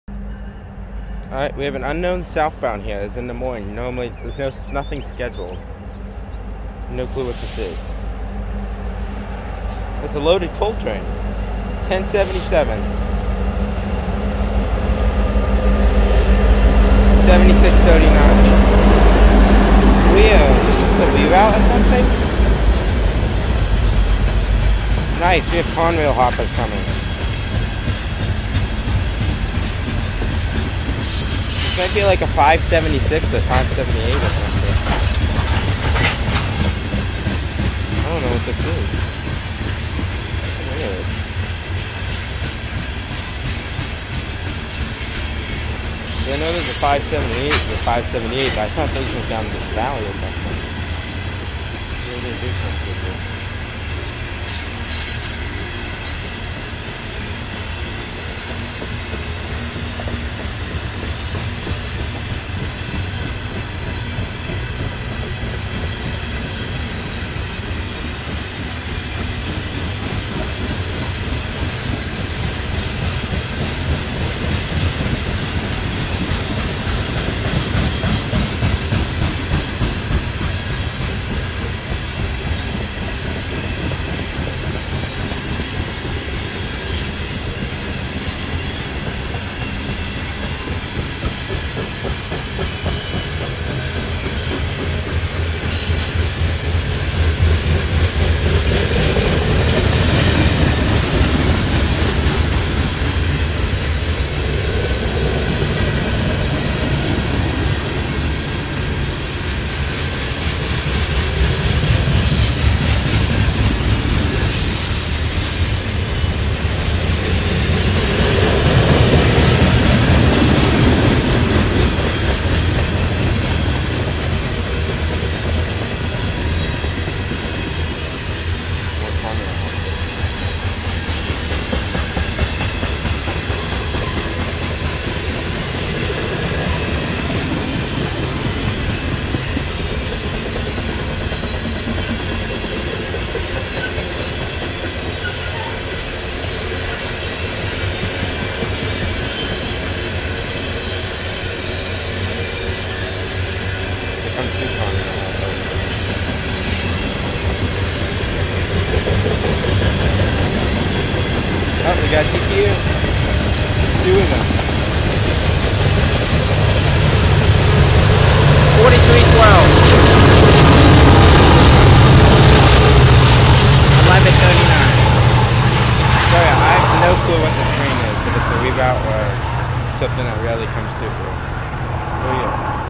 NS 576 reroute in Charlottesville Virginia
NS 576 is a rare loaded coal train that occasionally runs. There were 6 Conrail hoppers in the mix.

Train info
Symbol: NS 576
Type of train: Loaded Coal
Date and time: 4/23/21 11:53 am
Origin: Shire Oaks, PA
Destination: Norfolk, VA
Frequency: As needed
Car count: 105
Power
NS 4312 GE AC44C6M (DPU)
NS 1139 EMD SD70ACe (DPU)